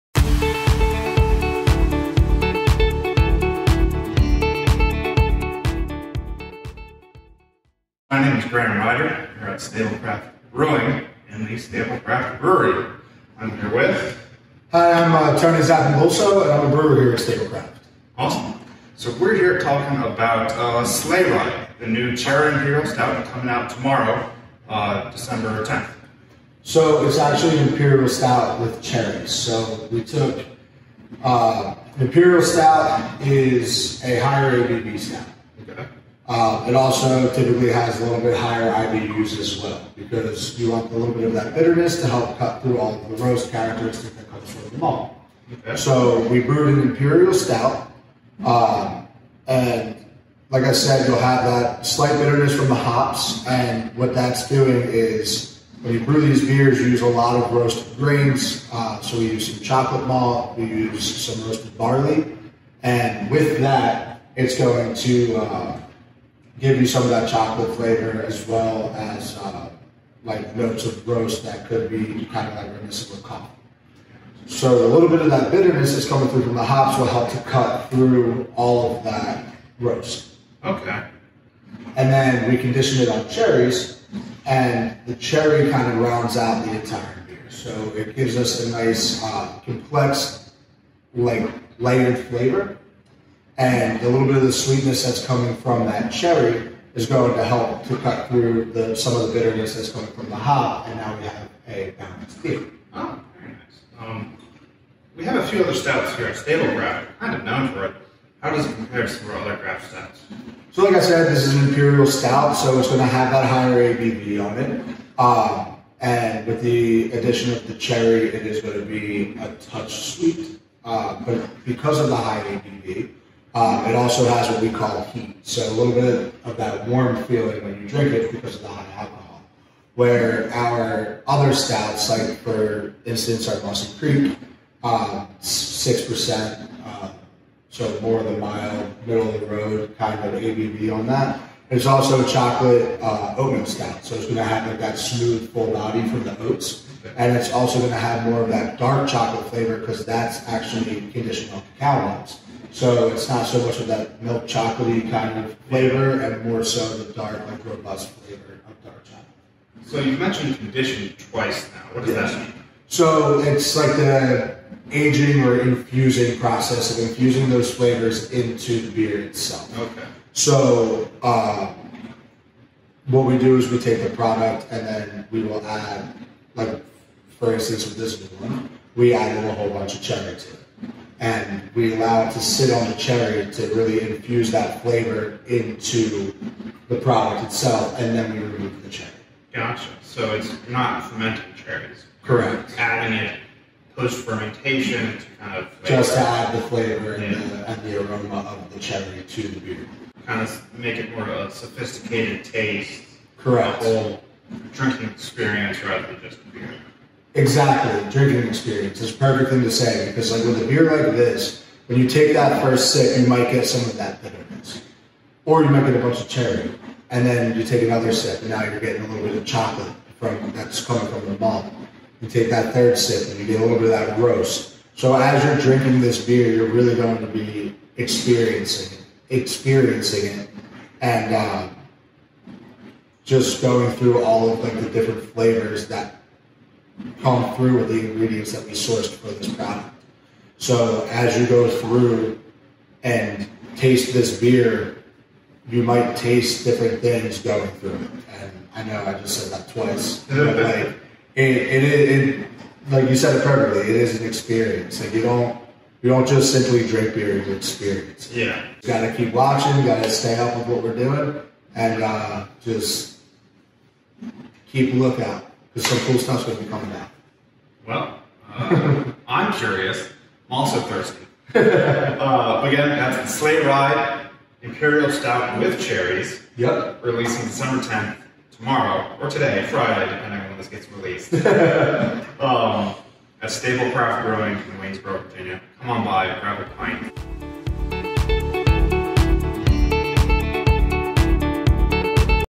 Whats Brewing at Stablecraft? December 10th.
Today we talk about Sleigh Ride our newest offering. It is an Imperial Stout with cherries.